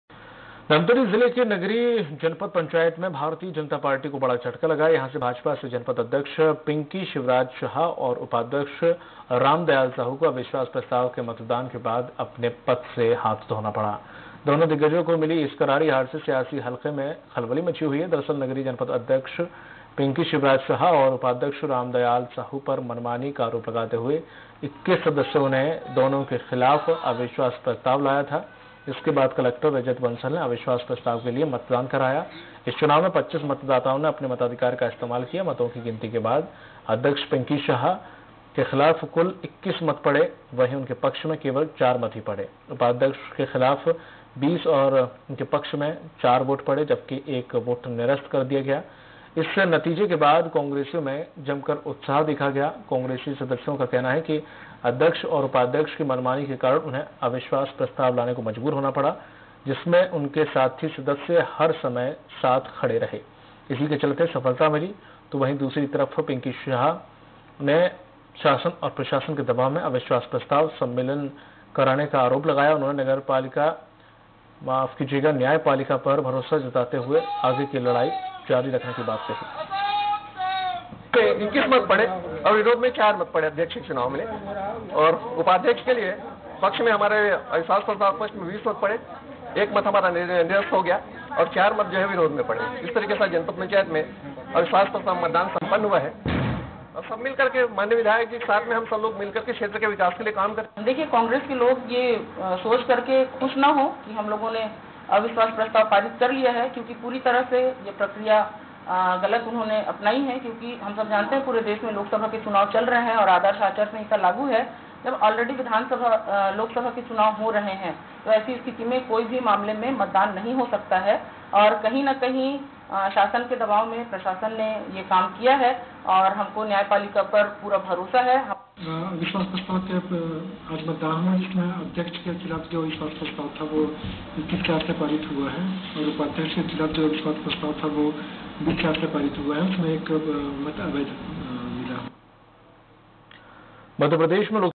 भाजपा के हाथ से फिसली नगरी जनपद अध्यक्ष और उपाध्यक्ष की कुर्सी
भाजपा के हाथ से फिसली नगरी जनपद अध्यक्ष और उपाध्यक्ष की कुर्सी, अविश्वास प्रस्ताव में 21 जनपद सदस्यों ने किया मतदान…